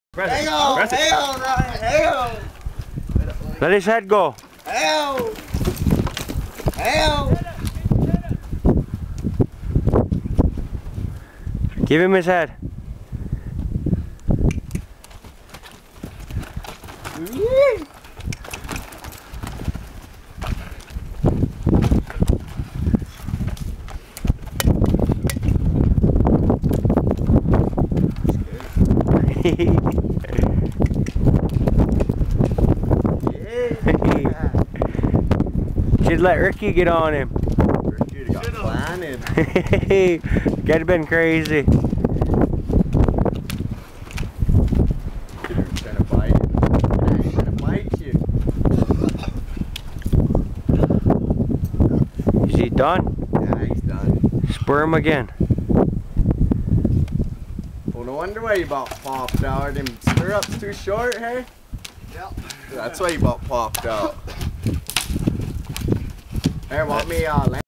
Riding a horse for the first time
This is the first time this horse has ever had a rider. The neck whipping is just to teach it how to turn.